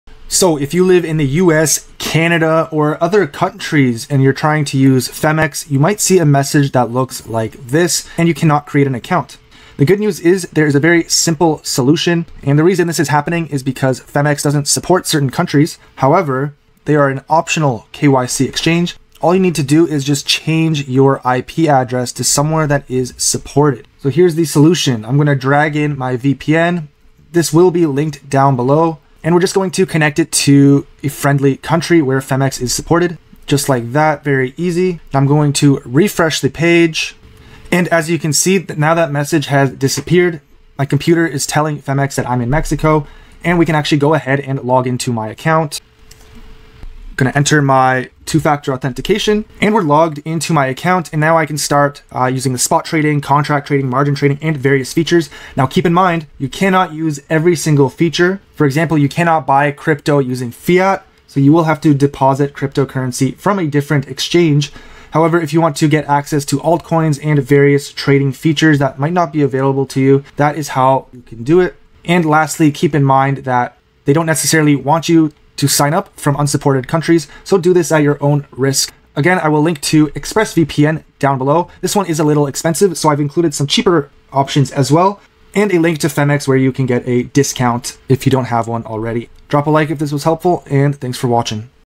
How to Access Phemex from Anywhere (Easy Fix)
Phemex not working? To use Phemex in the US, Canada, UK and other countries, there is a simple solution. Here's everything you need to know. Resources mentioned ⤵️

-- 3. ExpressVPN (VPN that I used)